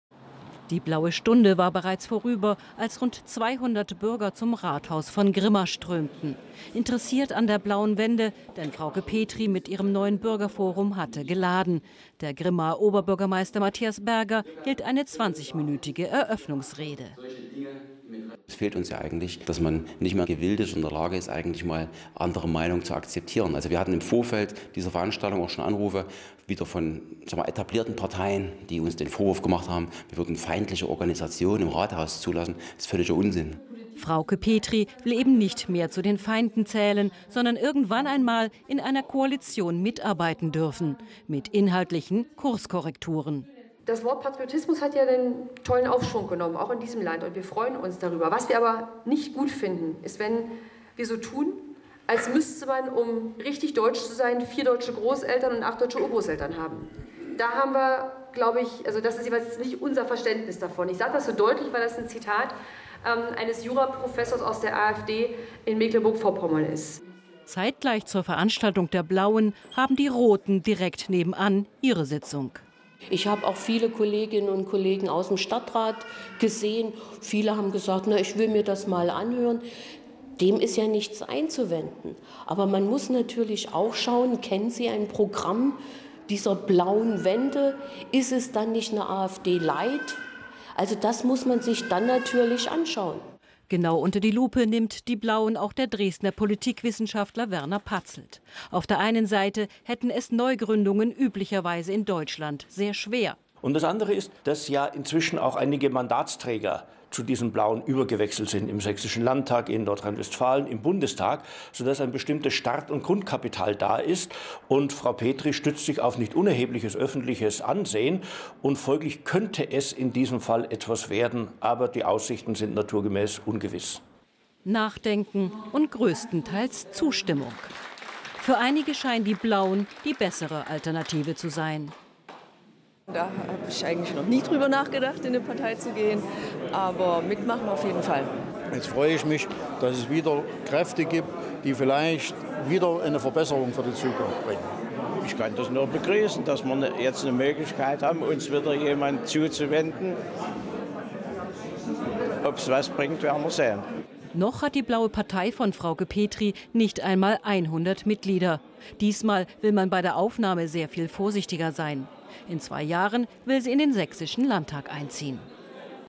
Blaue Wende statt AfD in Grimma
Die Blaue Wende um Frauke Petry informierte am 28.11.17 über Ihren Weggang aus der AfD.